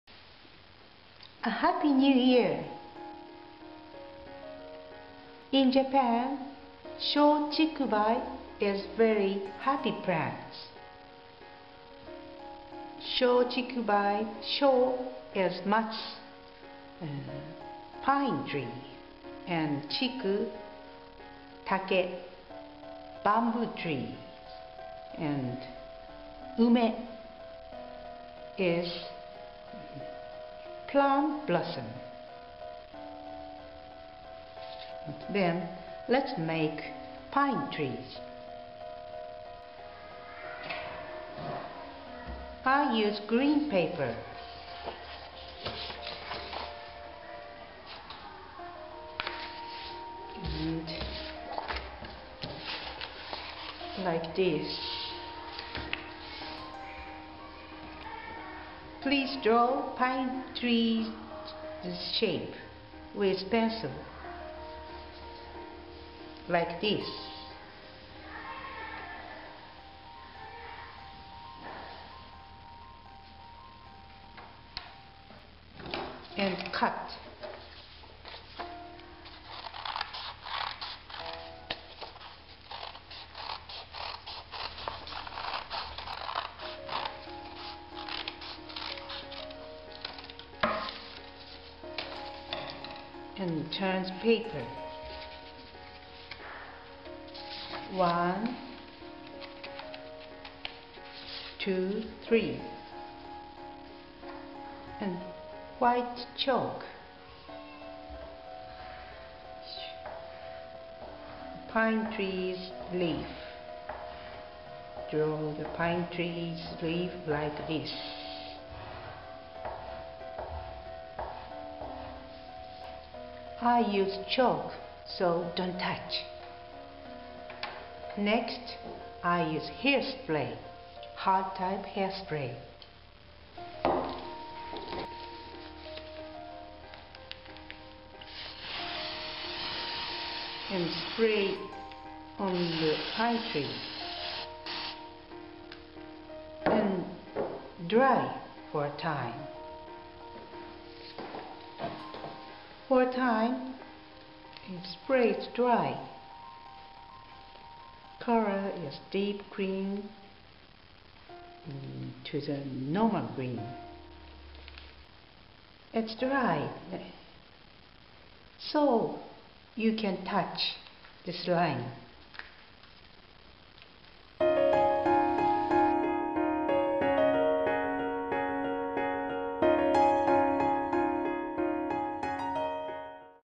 kimie gangi How to make a pine decoration with green pape
’Sho-chiku-bai(pine, bamboo, and plum)' is a lucky symbol for a new year.
Another way to read “sho” is “matsu”. It means pine trees.
Let's make a pine tree with green paper.